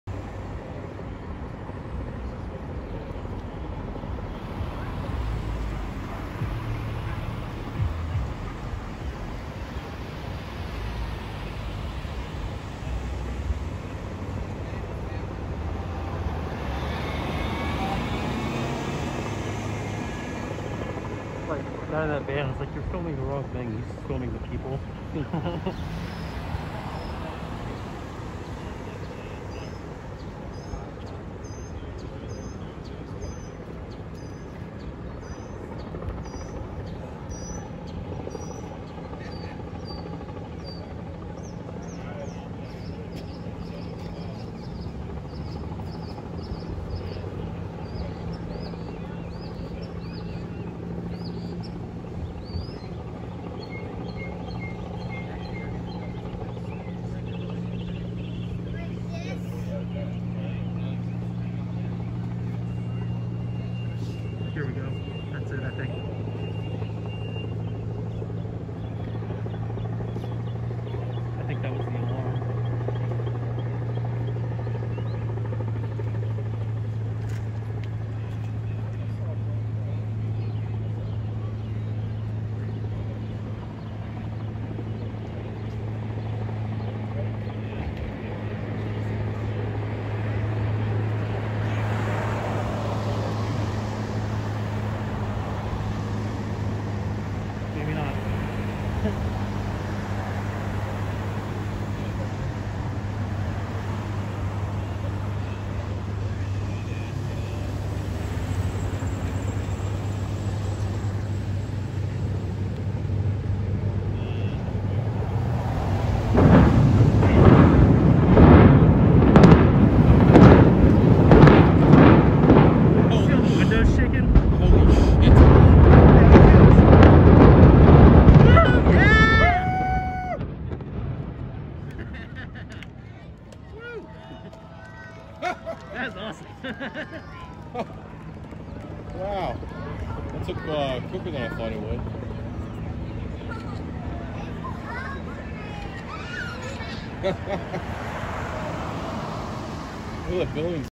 Dominion Energy building demolition
Implosion of the old Dominion energy office building in Richmond Virginia May 30, 2020